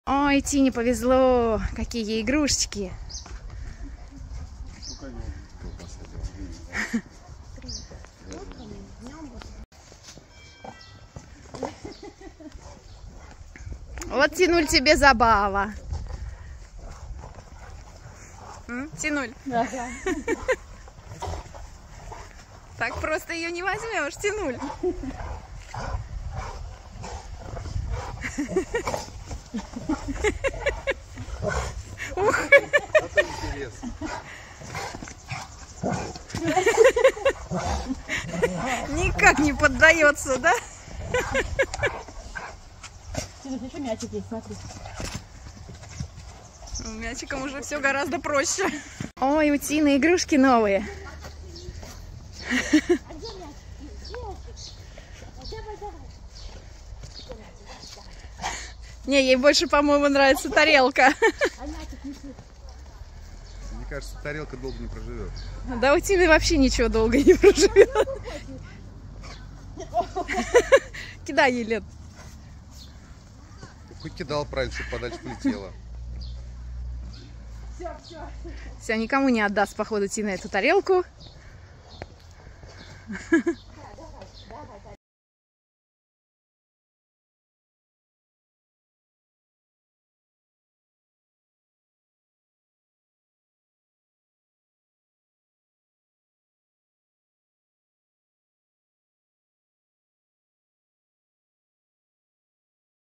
Ротвейлер Тина и гости
С приездом гостей сильно подфортило ротикоманда_Тина, привезли новые игрушки и ей выпала честь опробовать!